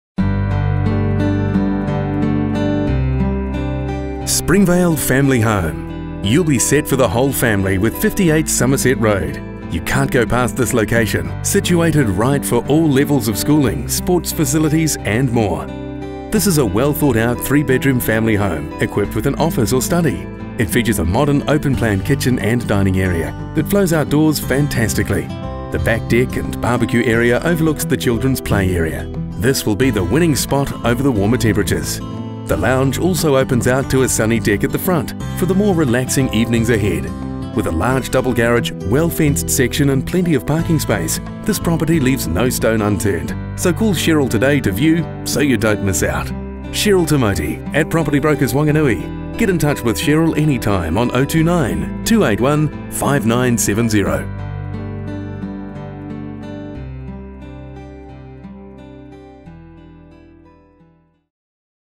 58 Somerset Road, Springvale.
Springvale Family Home

You will be set for the whole family, with 58 Somerset Road. You cannot go past this location, situated right for all levels of schooling, sports facilities, and more.

This is a well thought out  three bedroom, family home, equipped with an office or study. It features a modern, open plan kitchen and dining area, that flows outdoors fantastically. The back deck, and barbeque area, overlooks the children’s play area. This will be the winning spot to be over the warmer temps.

The lounge, also opens out to a sunny deck, at the front, for the more relaxing evenings ahead.

With a large double garage, well fenced section, and plenty of parking space, this property leaves no stone unturned. Phone Cheryl today to view so you don't miss out.

Cheryl Timoti at  Property Brokers, Wanganui.   Get in contact with Cheryl anytime on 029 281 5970